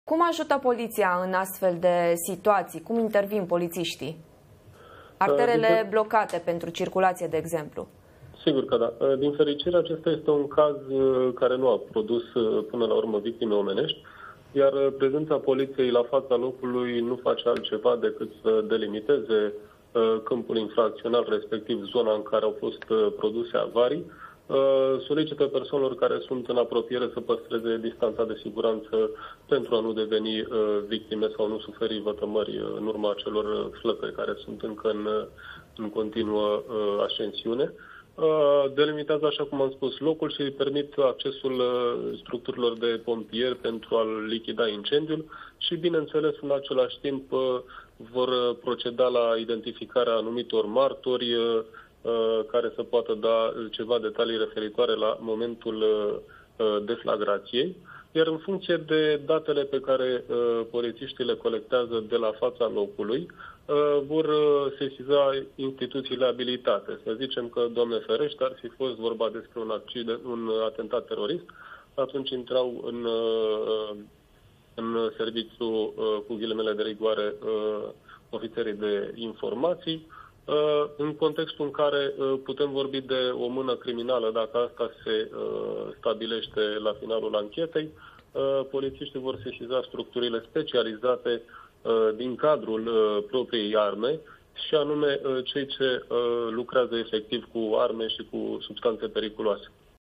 Dan Saltelechi, sindicalist poliție, despre explozia de la metroul din Londra
Dan Saltelechi, sindicalist poliție: „În contextul în care putem vorbi de o mână criminală, dacă asta se stabilește la finalul anchetei, polțiștii vor sesiza structurile specializate din cadrul propriei arme”